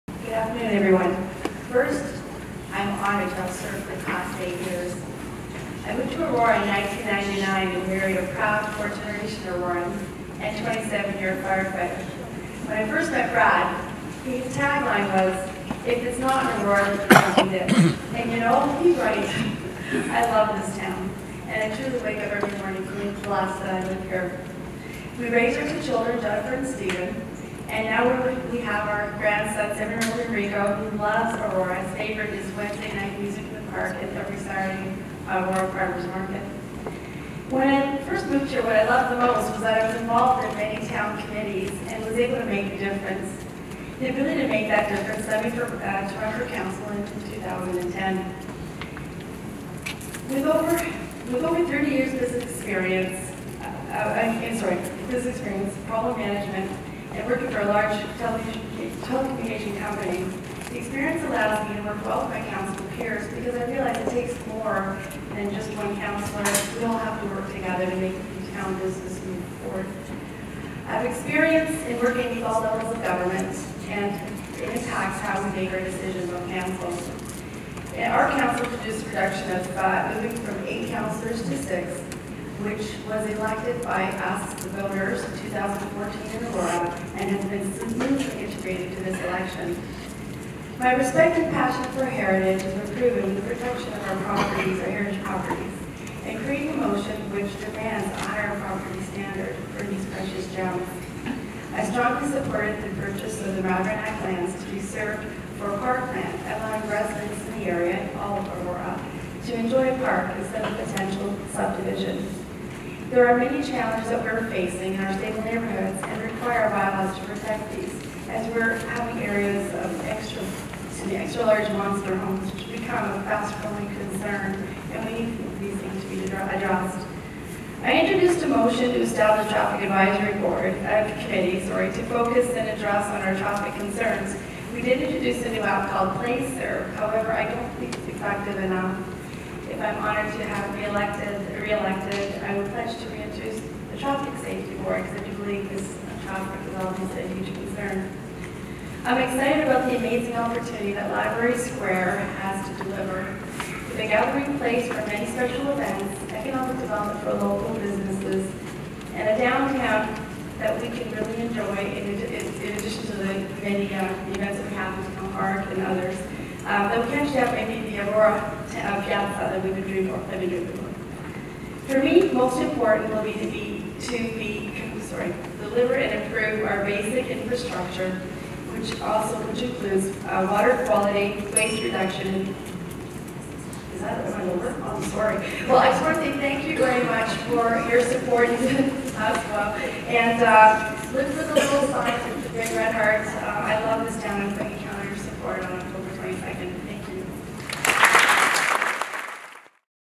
Sandra Humfryes for Aurora Council, APL All Candidates Meeting, Municipal Elections 2018
Municipal Election: All Candidates Meeting
September 30th 2018, 2:00pm to 4:30pm
Aurora Municipal Elections 2018 All Candidates Meeting organized by the Aurora Public Library. All registered candidates for the offices of Mayor, Councillor and School Board Trustee in the Town of Aurora. Meeting held at Aurora Town Hall, Council Chambers, 100 John West Way.